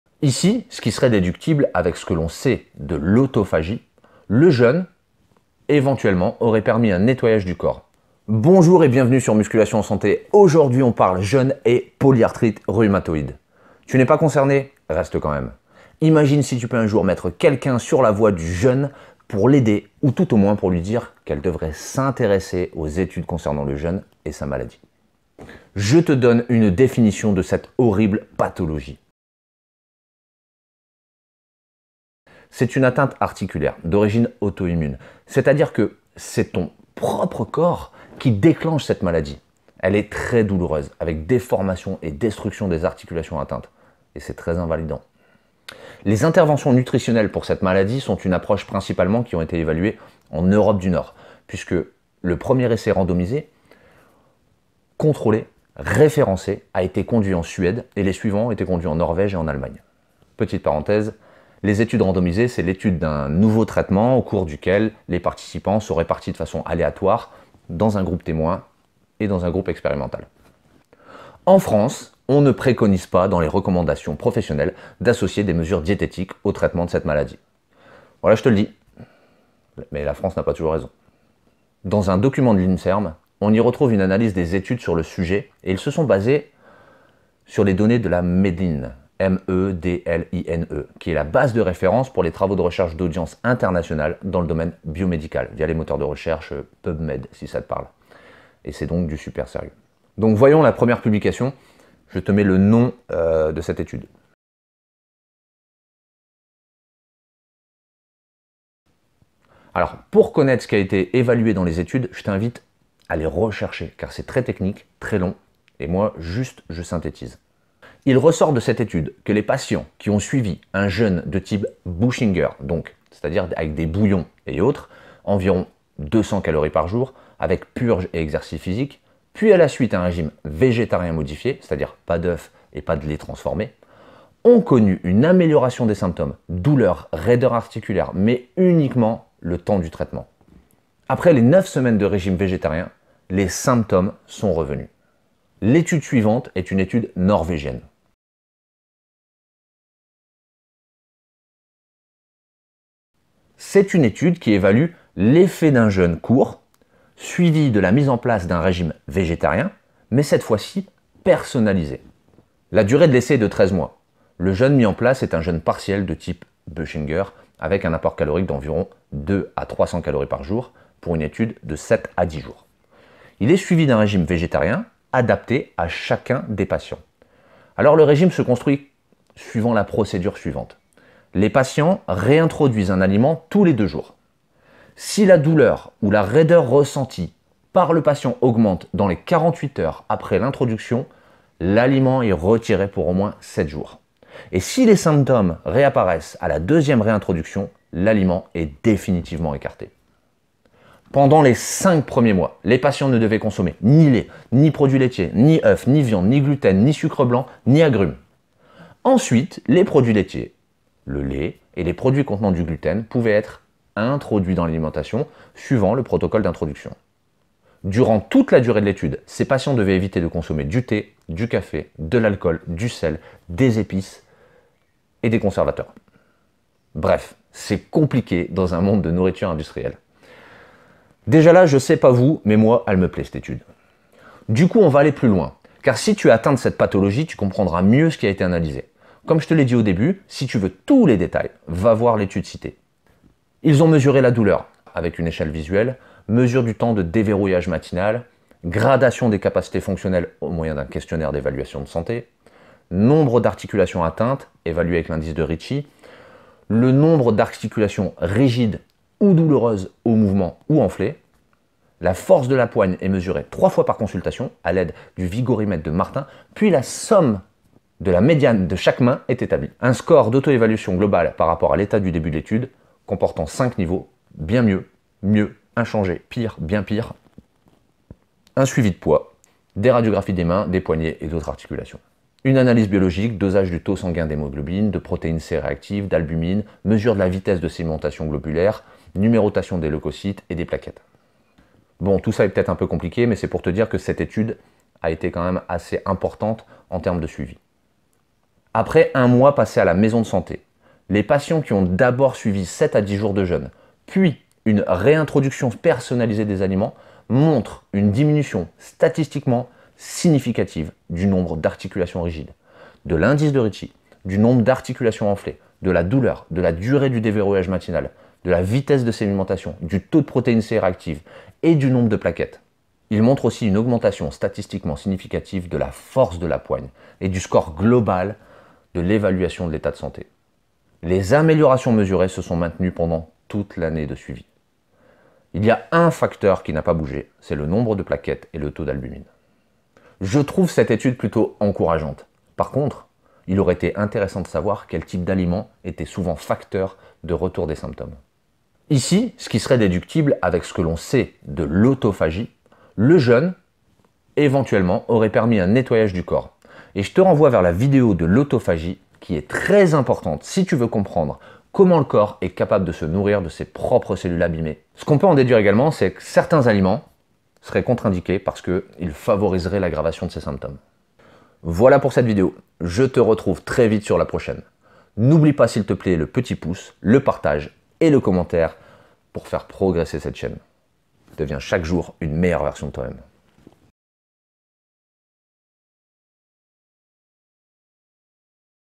Soigner la polyarthrite rhumatoïde avec le jeûne
Aujourd'hui on parle Jeune et polyarthrite rhumatoïde.
Tu n’es pas concerné? Reste quand même, imagine si tu peux un jour mettre quelqu’un sur la voie du jeûne pour l’aider ou tout au moins lui dire qu’elle devrait s'intéresser aux études concernant le jeûne et sa maladie.

Les interventions nutritionnelles pour cette maladie sont une approche principalement évaluée en Europe du nord, puisque le premier essai randomisé contrôlé référencé a été conduit en Suède et les suivants ont été conduits en Norvège et en Allemagne.
En France, on ne préconise pas dans les recommandations professionnelles d’associer des mesures diététiques aux traitement de cette maladie.